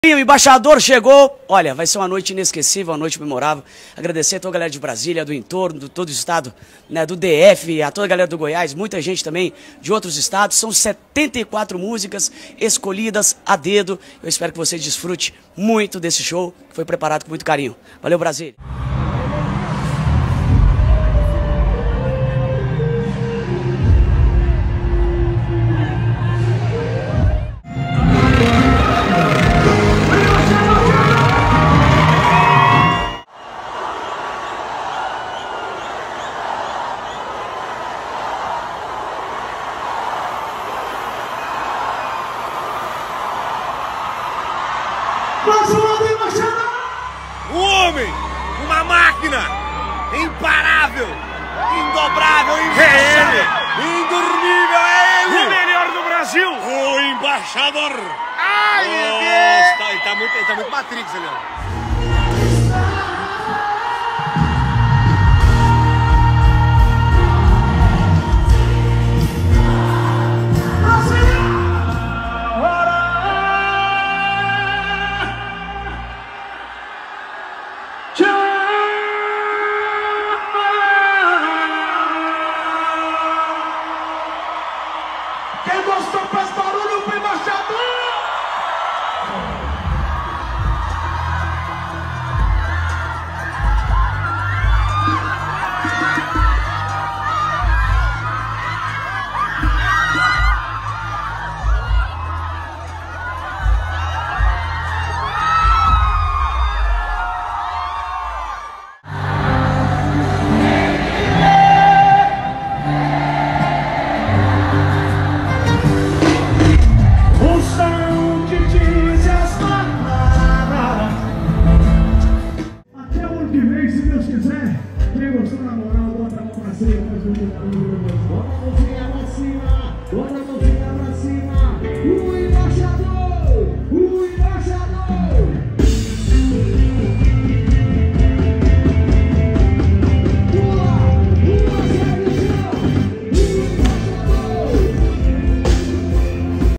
Gusttavo Lima no Buteco em Brasília 2023 (Melhores Momentos resumão)
Gusttavo Lima encantou o público de Brasília com um show memorável no maior Buteco do mundo, o embaixador cantou 74 músicas durante a apresentação e marcou história mais uma vez na cidade